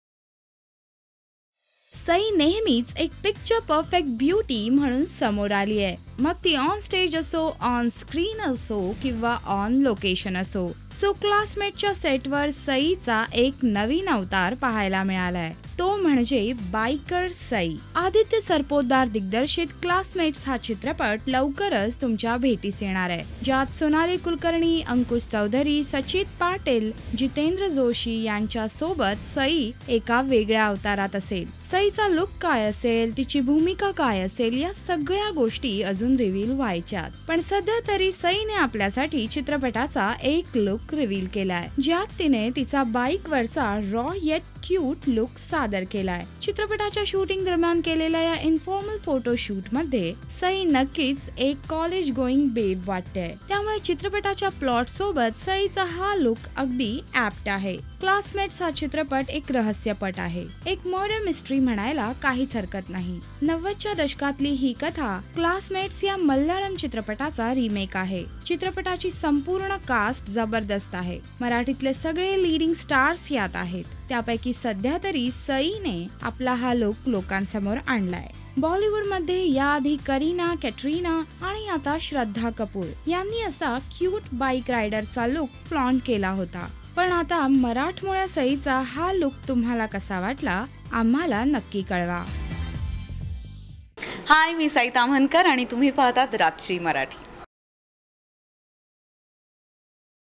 Sai Tamhankar Learns Bike Riding For Her Next Marathi Movie - Classmates!!!
Sai Tamhankar Learns Bike Riding For Her Next Marathi Movie!! Beautiful & sexy Sai Tamhankar is ready to reveal her adventurous side for all her fans. She is learning to ride a bike for her next marathi movie "Classmates" starring Ankush Choudhary, Sonalee Kulkarni, Jitendra Joshi among others. Watch this video on Rajshri Marathi!